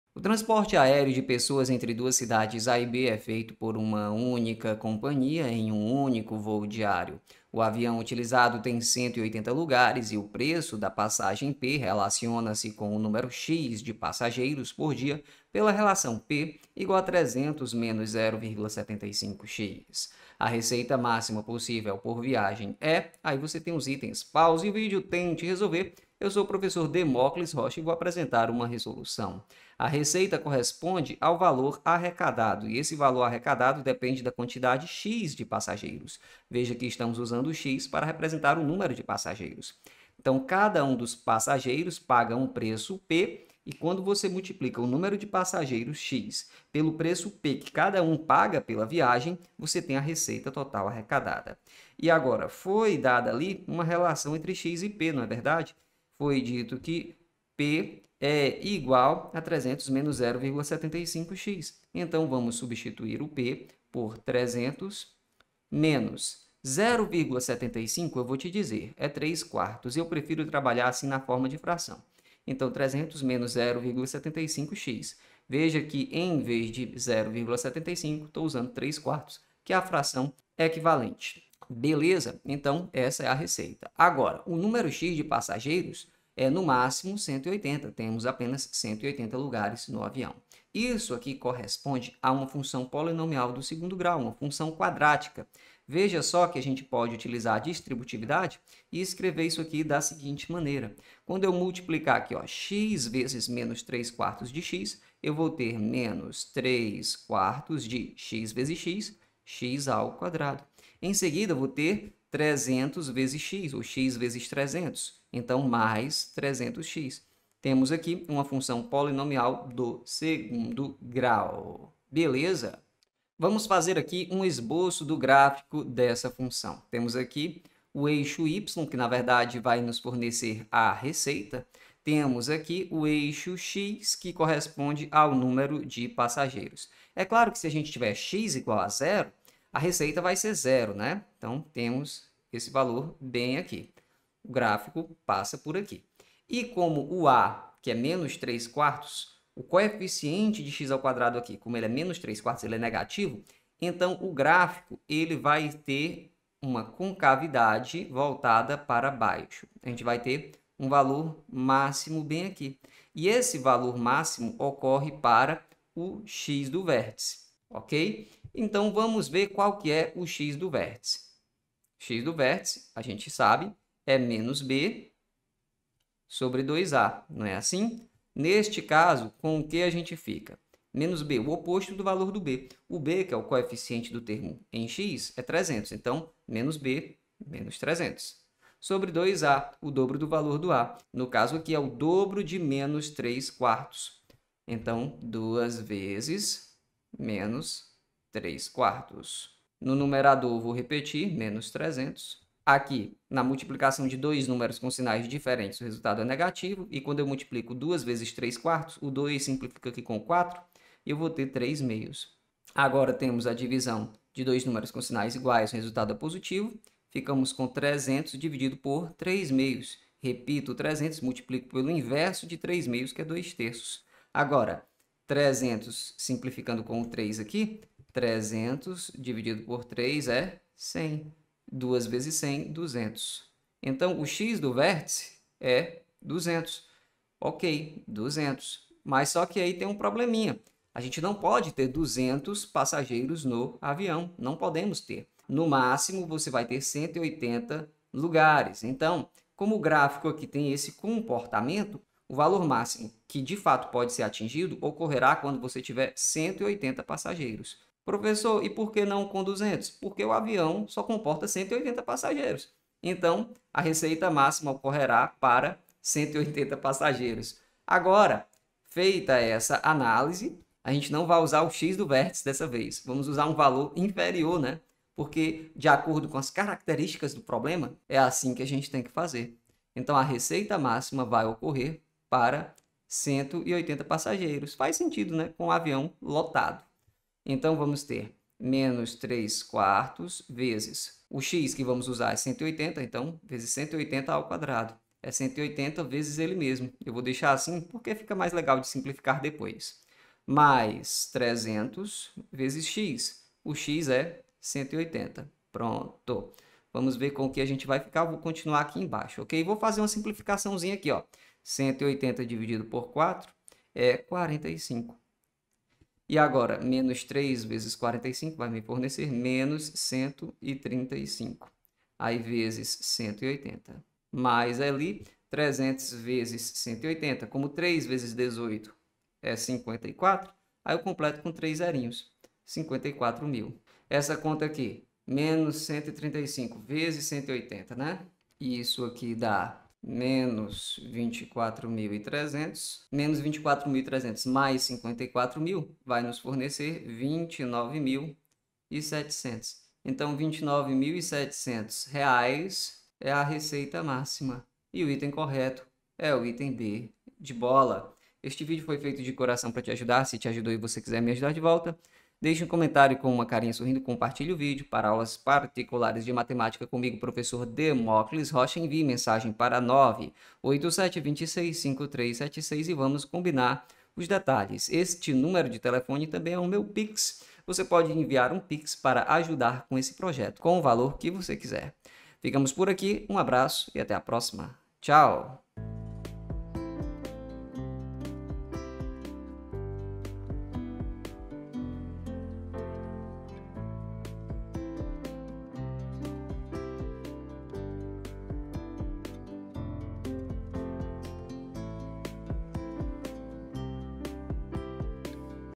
RECEITA MÁXIMA
Neste vídeo, apresento uma resolução para uma questão sobre valor máximo de uma função quadrática.

Envie uma mensagem para a gente combinar o valor e agendar.